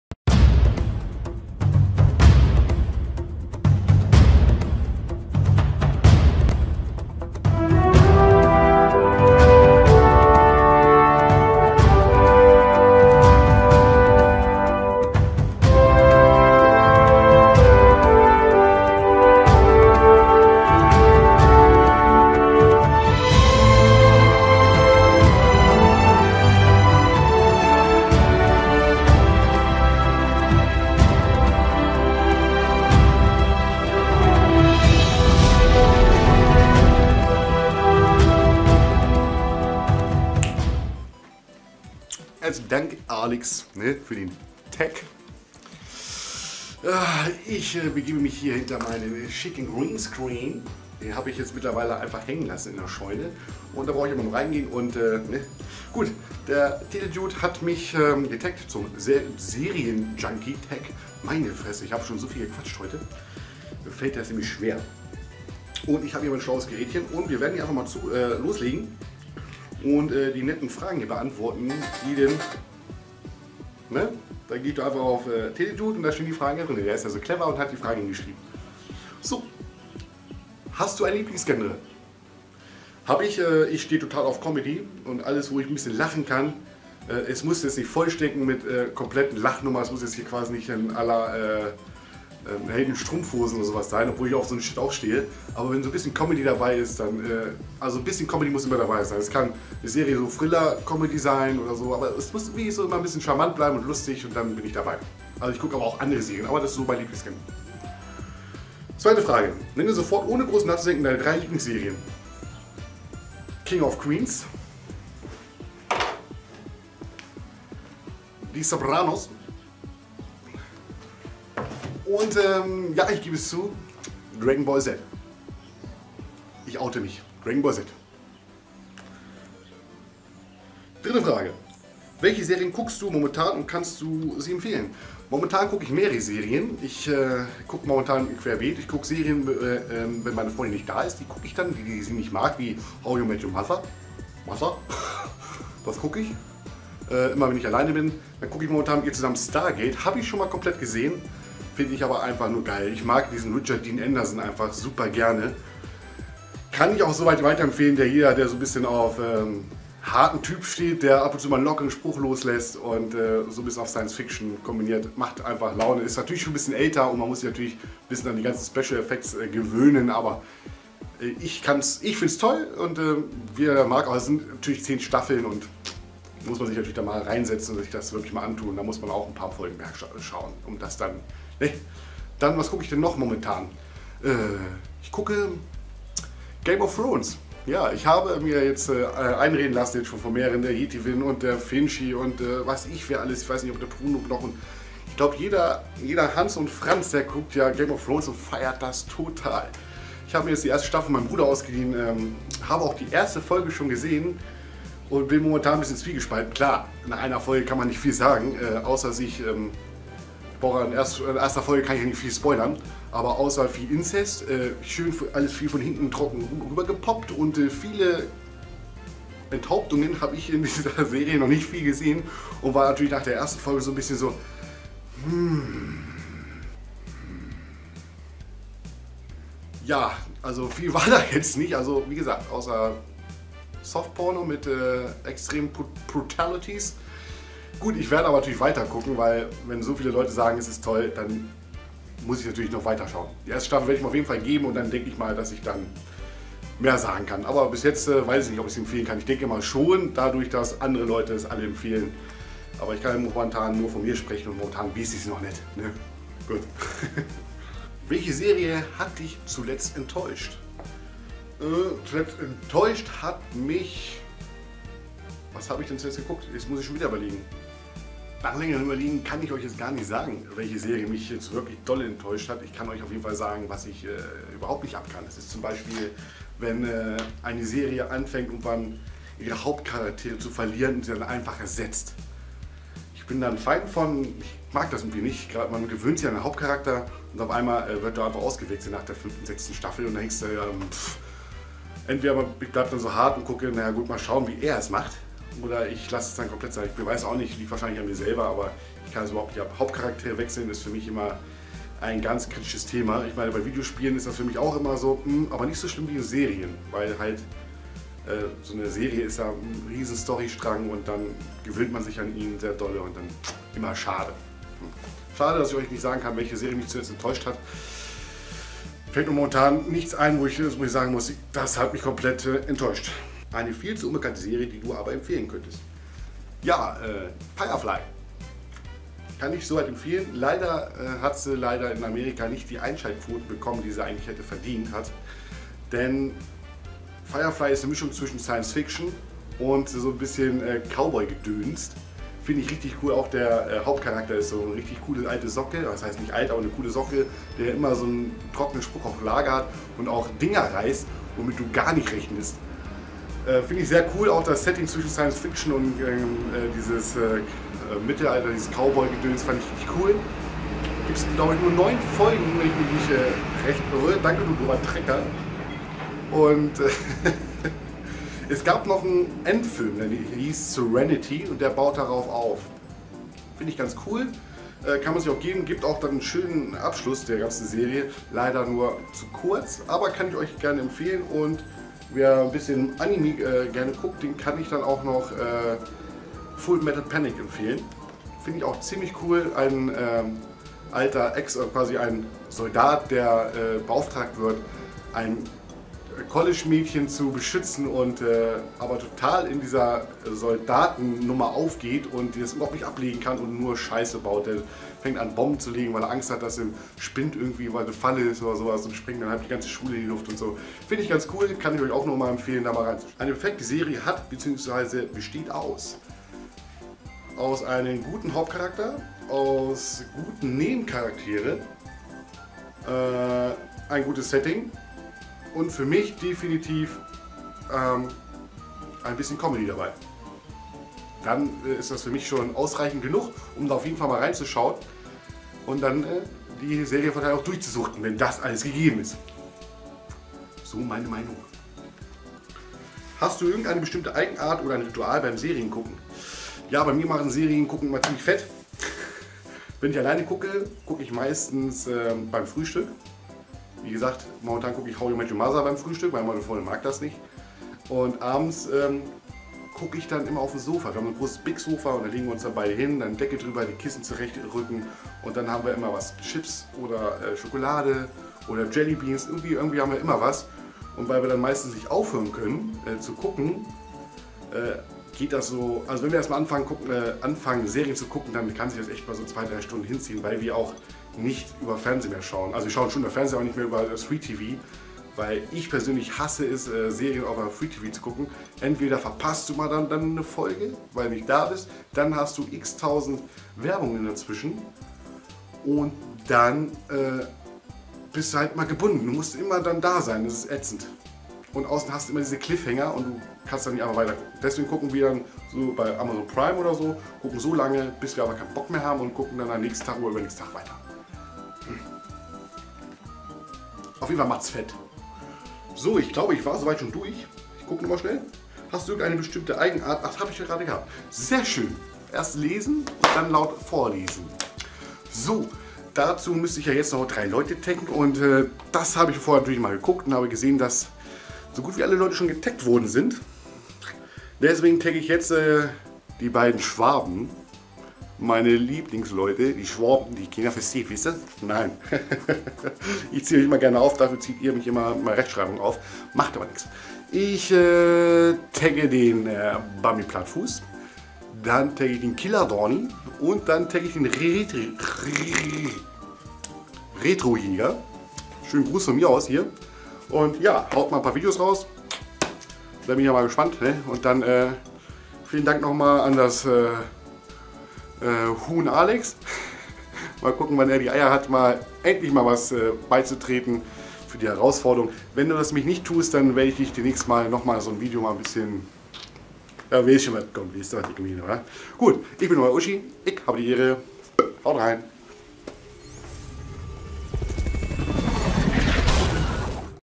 GC Serien Junky tag
Getagt vom Tele DUDE.
Da mach ich natürlich mit .Die erste Frage aus Absicht ausgelassen, da man sie nicht richtig beantworten kann .

Die fragen:
1. Wie viele Serien hast du bisher schätzungsweise geschaut?
2. Hast du ein Lieblingsgenre?
3. Nenne sofort ohne groß nachzudenken deine drei       Lieblingsserien!
4. Welche Serie(n) kuckst du momentan und kannst du sie empfehlen?
5. Welche Serie hat dich zuletzt enttäuscht?
6. Eine viel zu unbekannte Serie, die du aber empfehlen könntest?
7. Eine perfekte Serie hat bzw. besteht aus...?
8. Hast du irgendeine bestimme Eigenart oder ein Ritual beim Serien gucken?